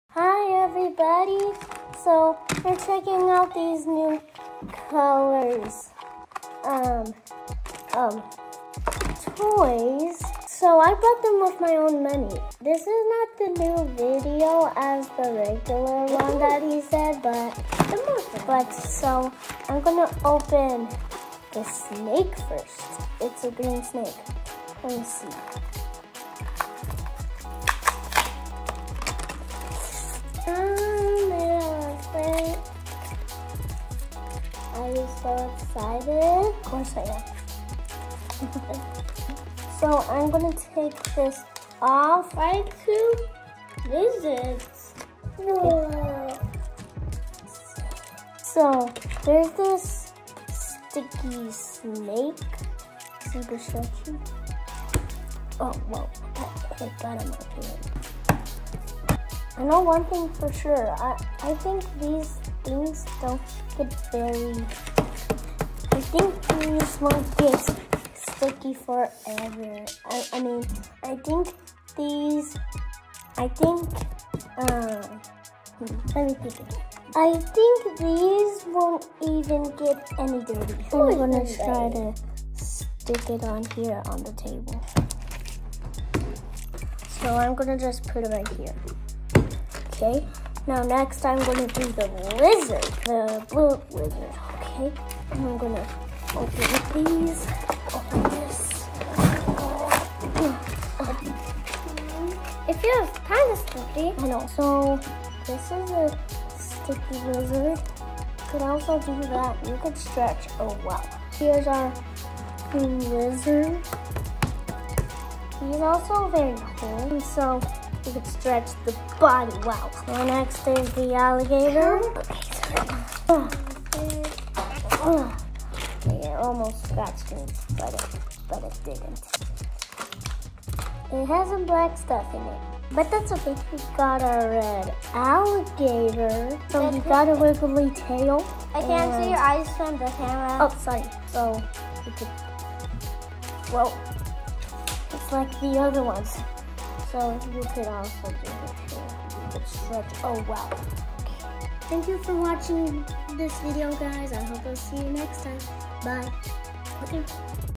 Stretchy Sticky Reptiles - Super Toys with Brady Boy
Hi guys! Today I want to show you my new stretchy, squishy, sticky toys from Amazon! They are only $13 and I love them!

Brady Boy